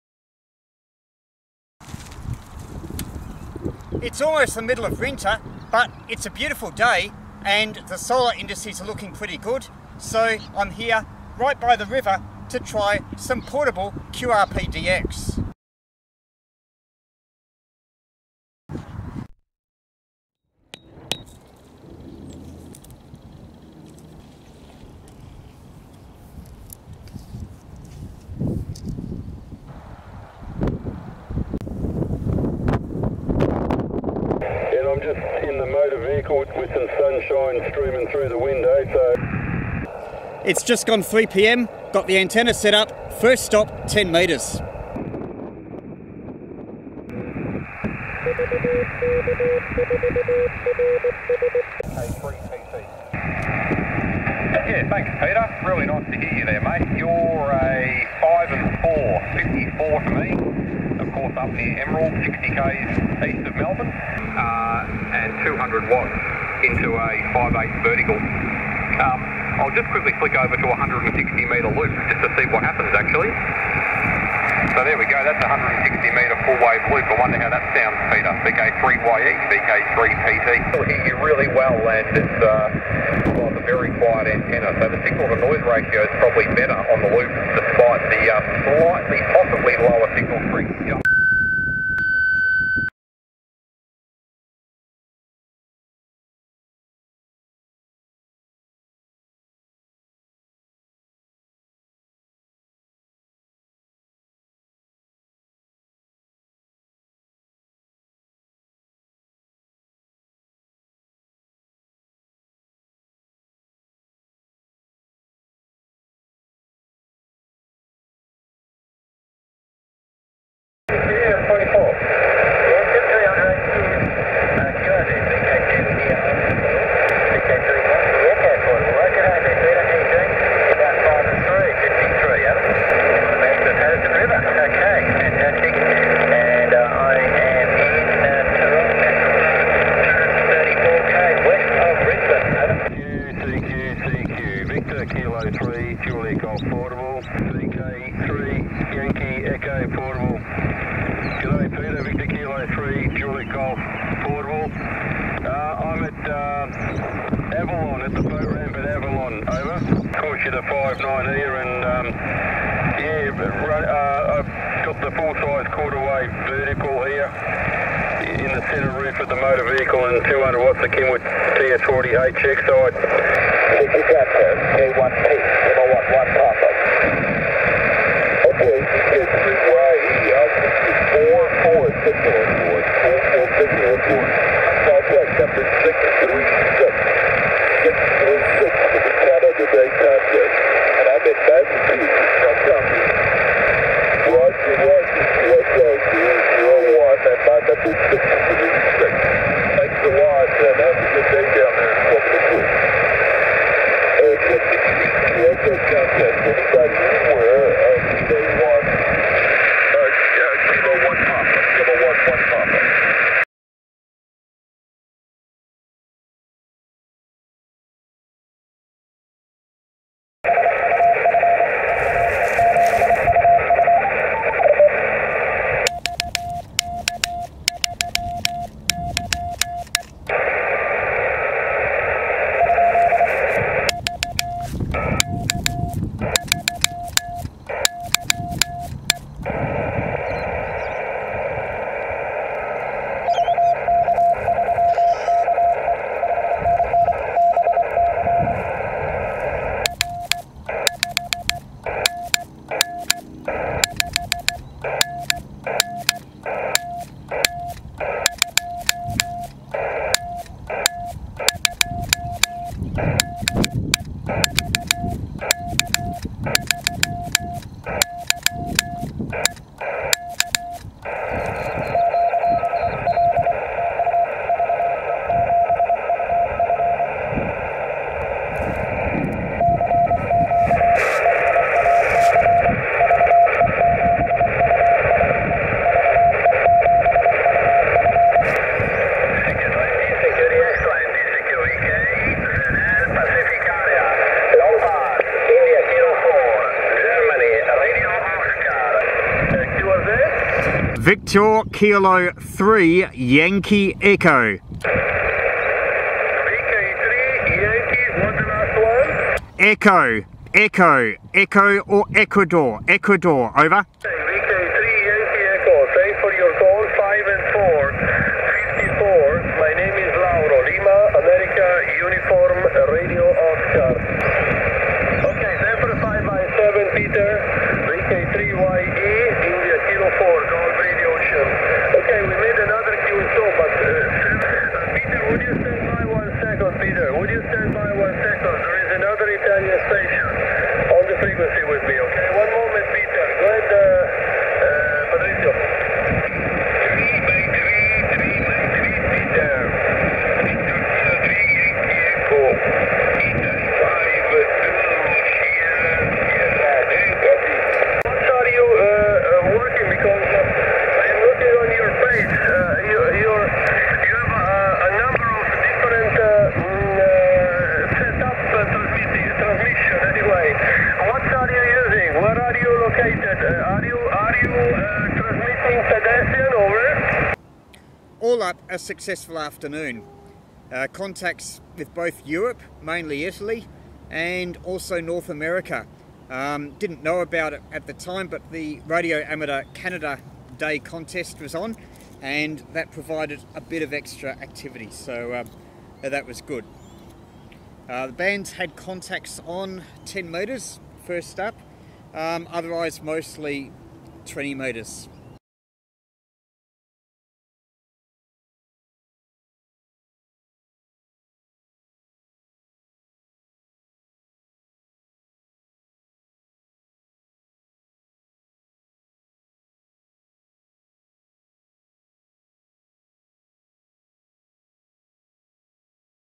QRP by the River 1 July 2021 inc Canada Day
Some portable QRP near home. Coincided with Canada Day contest. Contacts on 10 & 20m.